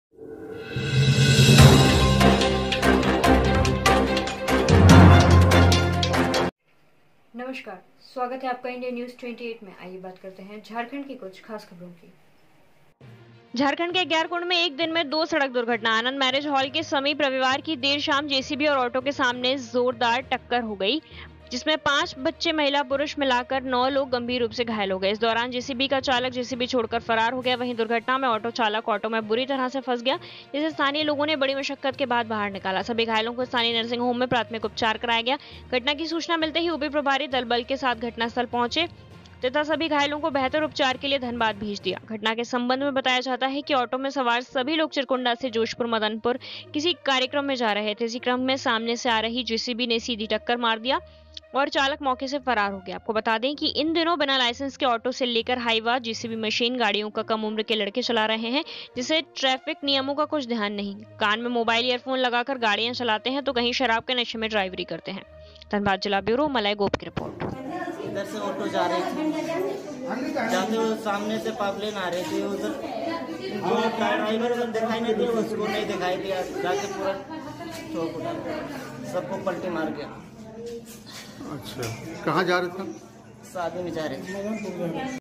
झारखंड के एग्यारकुण्ड में एक दिन में दो सड़क दुर्घटना
Official